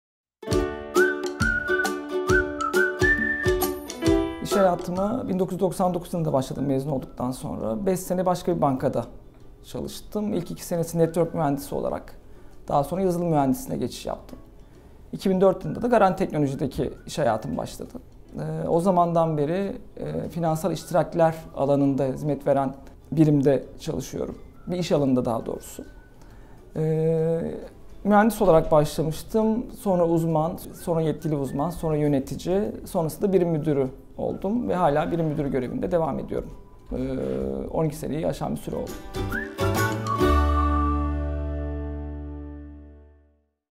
Garanti BBVA Teknoloji'de hikayeniz nasıl başladı? Bugüne kadar hangi görevlerde bulundunuz?
Garanti Teknoloji'de hikayeniz nasıl başladı? Bugüne kadar hangi görevlerde bulundunuz?

Garanti Teknoloji Yazılım Birim Müdürü Ozan Solmaz Anlatıyor!